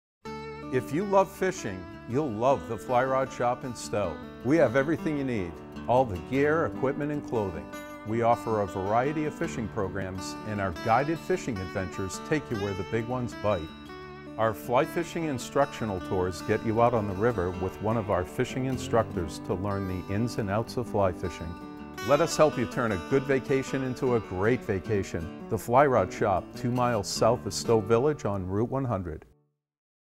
Half & Full Day Fly Fishing Instructional Tours
Our Daily Fly Fishing Instructional Tours are a unique program that start out by teaching you the roll cast & false cast on our stocked trout pond in Stowe, Vermont. Then our guides will outfit you in waders and boots to head to the river where we will teach you the elements of fly fishing by allowing "the river to become the classroom." Experienced guides then discuss fly presentation, how to mend the line, how to read the water, aquatic entomology, and then show you how to match the hatch.